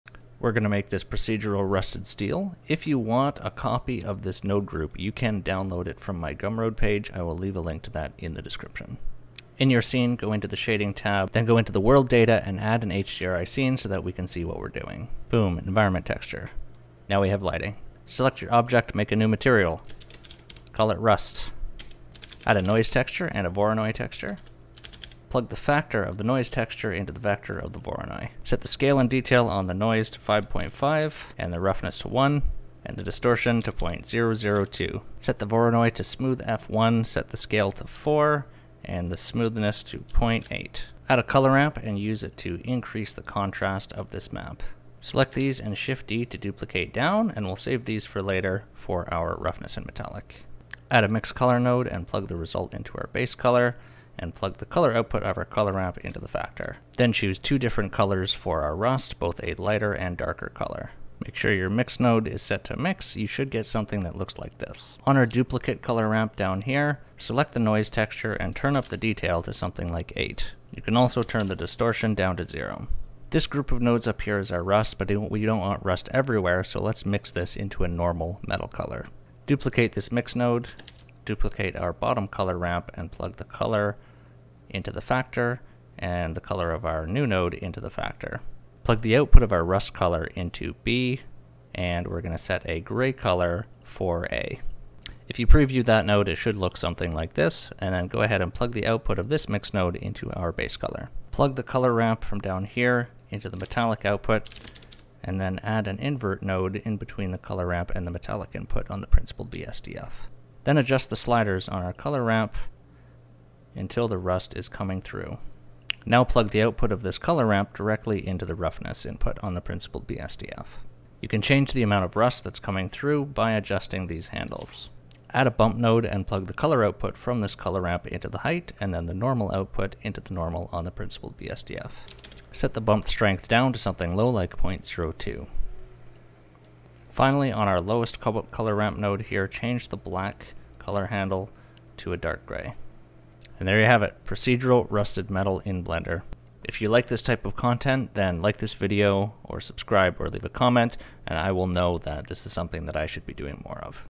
Blender: Easy procedural rusted metal material
How to make a simple procedural rusty steel metal material / textures in blender in under 3 minutes.